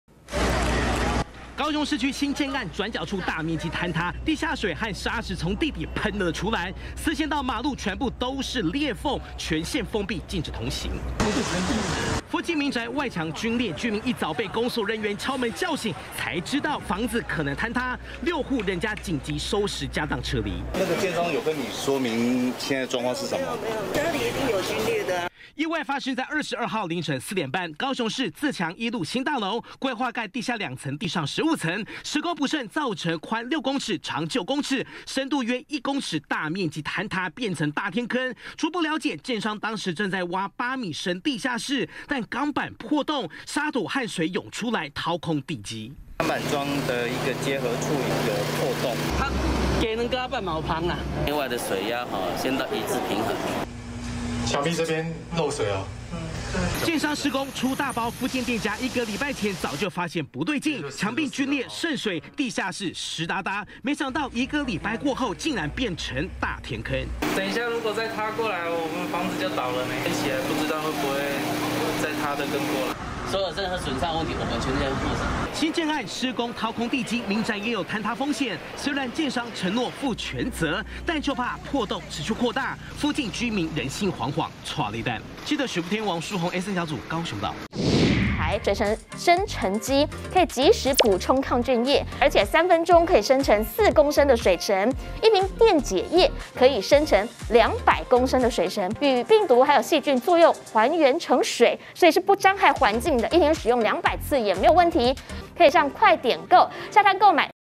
【每日必看】又見天坑! 新建案出包"掏空地基"路塌@中天新聞CtiNews 20220322
高雄市區自強一路，一處新建案工地，今天凌晨4點多，因為地下室"鋼板樁"破洞，地下水跟砂土湧出，掏空馬路地基，造成大面積坍塌，柏油路面嚴重扭曲變形，形成長9米、寬6米，深約1米的天坑，距離民宅短短2公尺，有傾倒風險，居民緊急撤離，就像在逃難，建商搶修，說雞蛋再密也會有縫隙，讓居民很不滿，上星期地下室就開始滲水，早有預兆，工務局到場，先勒令停工，並裁罰9萬元。